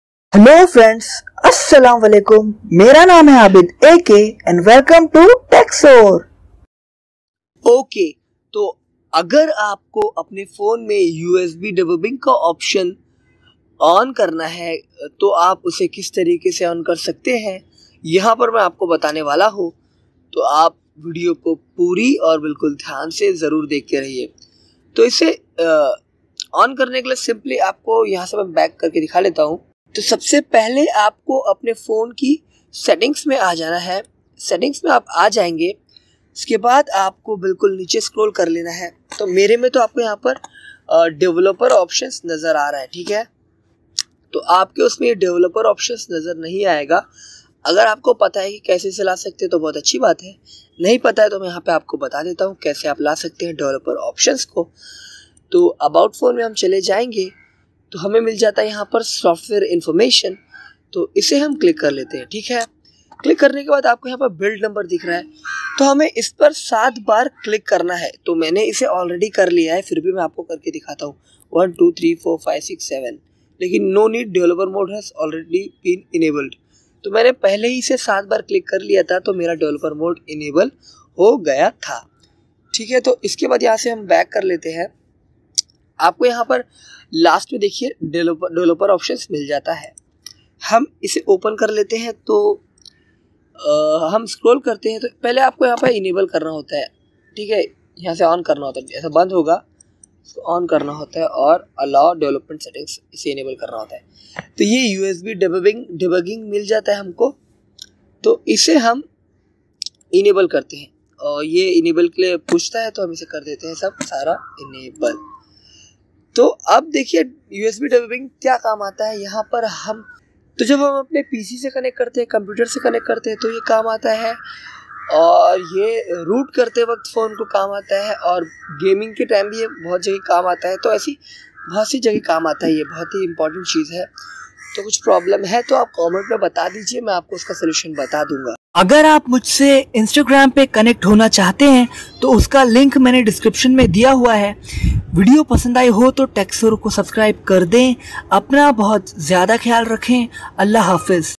All Samsung Galaxy Phones: Enable USB Debugging Mode (Developer Options Menu) A51
in you amdroid samsung phone Usb debugging is intended for development purpose only. Use it to copy data between your comouter and your device, it is very usefull install apps on your device without notification, and read log data.

Talk soon,
Abid Akay